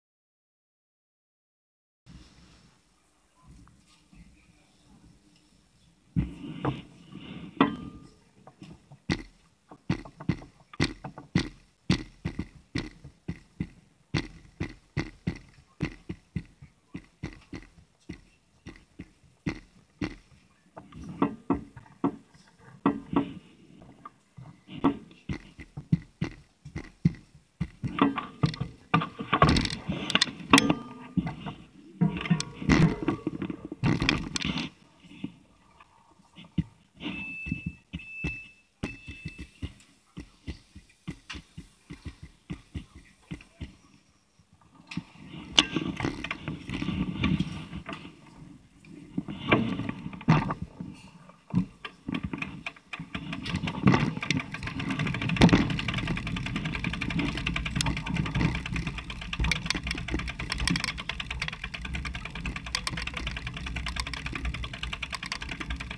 riding off the train
dude on his bike on a train. see how he dismounts...